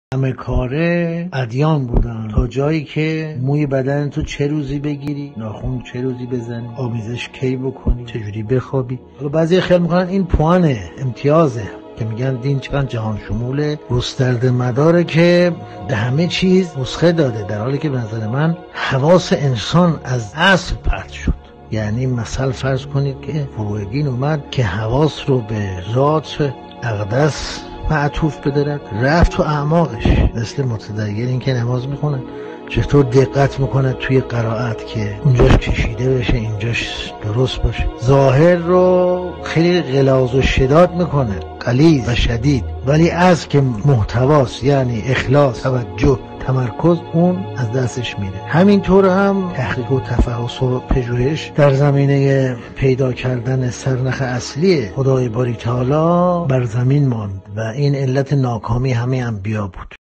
ترک معادلات تکراری 17
یک دقیقه همراه با بروجردی،
قسمت17: علت ناکامی انبیاء

ادیان در همه کارهای بشر وارد شدند! تا جایی که موی بدنت را چه روزی بگیری، ناخنت را چه روزی بزنی، چه موقعی آمیزش بکنی، چطور بخوابی! حالا بعضی‌ها خیال می‌کنند این یک امتیاز است و می‌گویند دین چقدر جهانشمول و گسترده است که برای همه چیز نسخه داده‌است، در حالی که به نظر من، حواس انسان از اصل پرت شد. مثل متدینی که نماز می‌خواند و در قرائت و نحوه ادای کلمات دقت بسیاری می کند و ظاهر کلمات را خیلی غلیظ و شدید می کند ولی اصل، که محتوا است یعنی اخلاص، توجه، تمرکز، از دستش می‌رود. همین‌طور هم تحقیق و تفحص و پژوهش در زمینه پیدا کردن سرنخ اصلیِ خدای باری‌تعالی بر زمین ماند و این علت ناکامی همه انبیاء بود